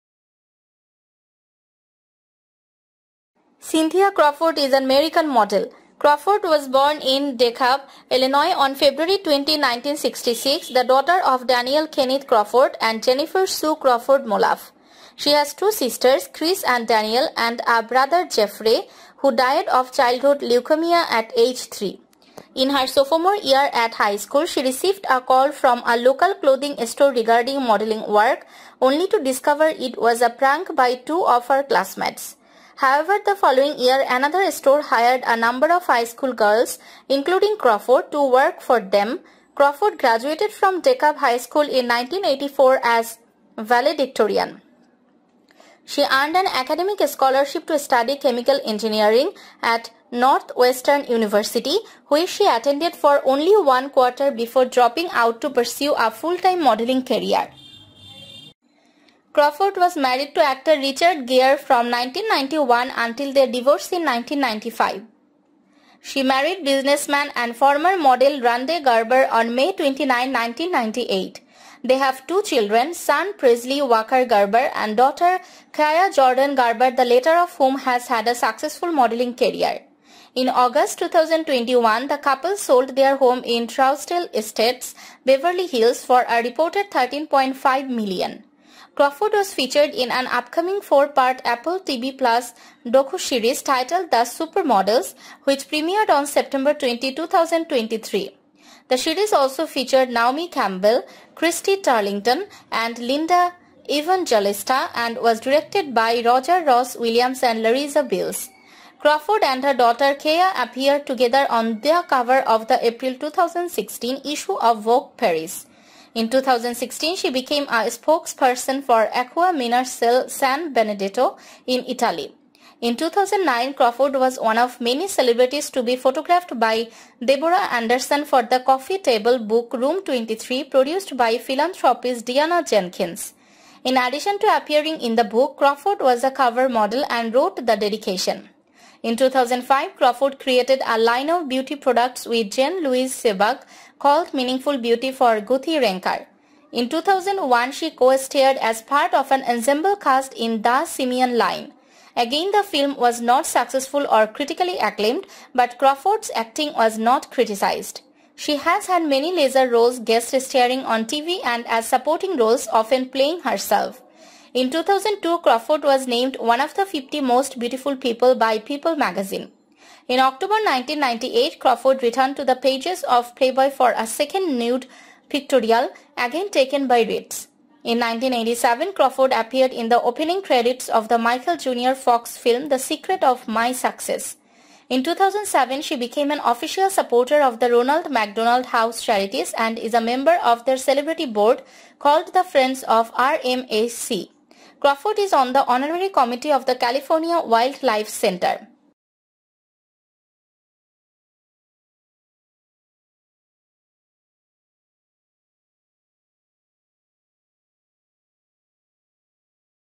Cindy Crawford || 10 Things You Need To Know About Cindy Crawford
Cynthia Crawford is an American model.
Crawford was born in DeKalb, Illinois, on February 20, 1966, the daughter of Daniel Kenneth Crawford and Jennifer Sue Crawford-Moluf .
She has two sisters, Chris and Danielle, and a brother, Jeffery, who died of childhood leukemia at age 3.
In her sophomore year at high school, she received a call from a local clothing store regarding modeling work, only to discover it was a prank by two of her classmates. However, the following year, another store hired a number of high school girls, including Crawford, to work for them.
Crawford graduated from DeKalb High School in 1984 as valedictorian.

Cindy Crawford
Cindy Crawford